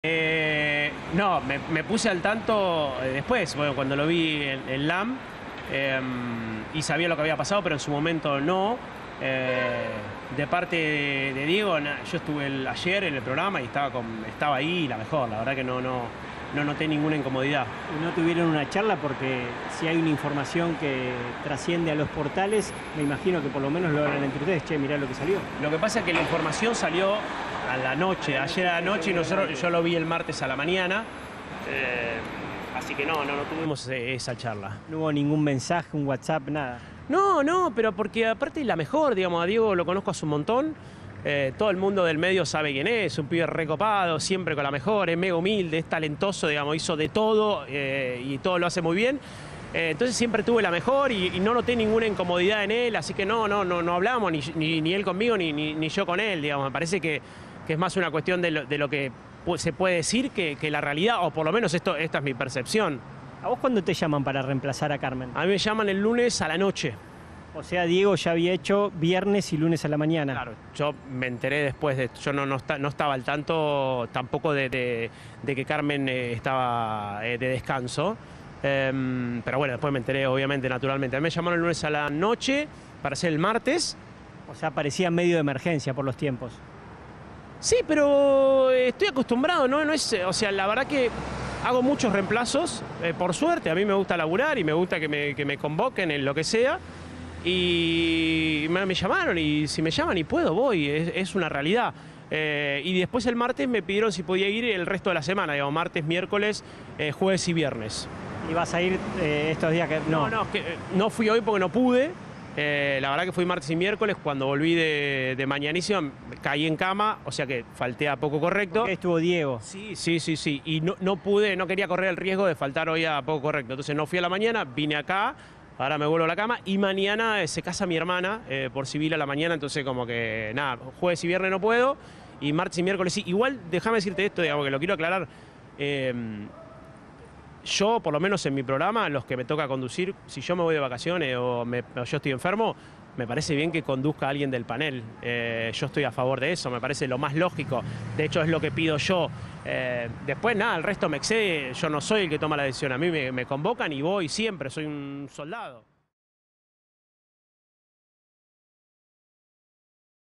🔥 EL POLLO ÁLVAREZ A FAVOR DE DIEGO RAMOS
"Estoy a favor de los panelistas como conductores".